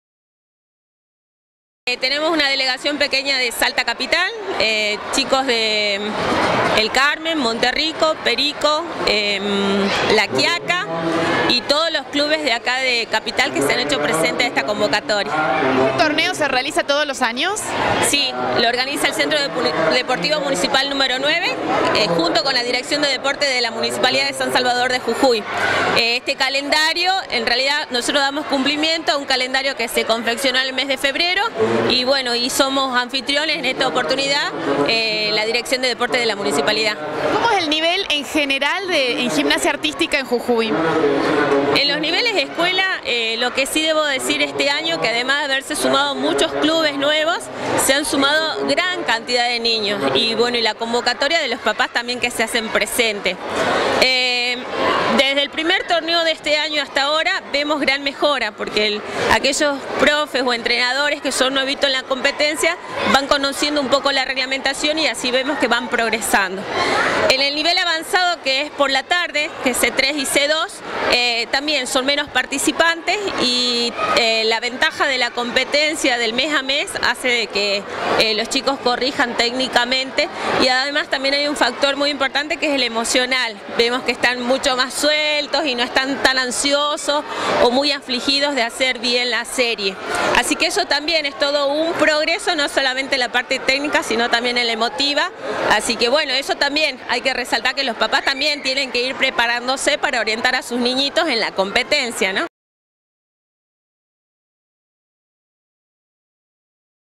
Torneo Provincial de Gimnasia Artística y Acrobática
El fin de semana se llevó adelante una nueva fecha del torneo en la Escuela Francesa con la masiva participación de referentes de la Provincia y el NOA.